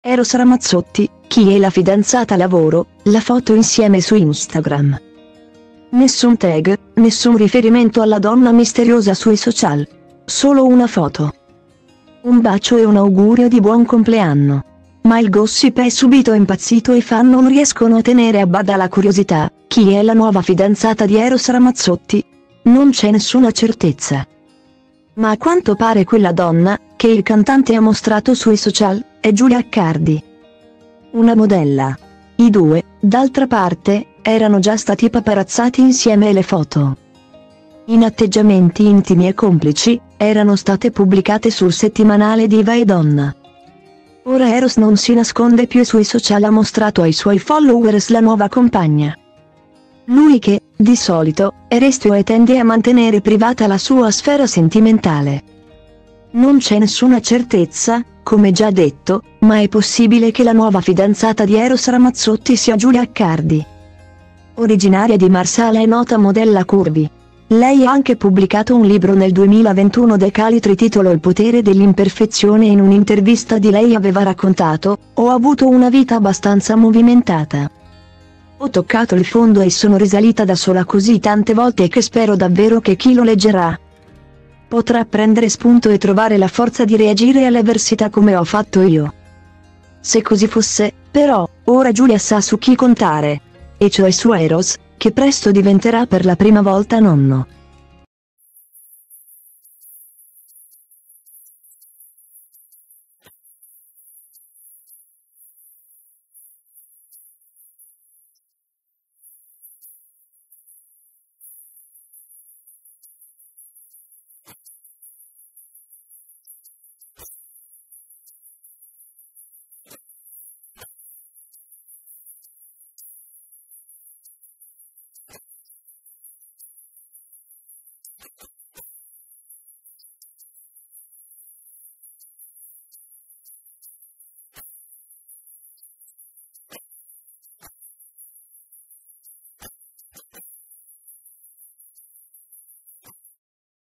Eros Ramazzotti, chi è la fidanzata? lavoro, la foto insieme su Instagram
Nessun tag, nessun riferimento alla donna misteriosa sui social. Solo una foto,un bacio e un augurio di buon compleanno. Ma il gossip è subito impazzito e i fan non riescono a tenere a bada la curiosità: chi è la nuova fidanzata di Eros Ramazzotti? Non c’è nessuna certezza,ma a quanto pare quella donna, che il cantante ha mostrato sui social, è Giulia Accardi,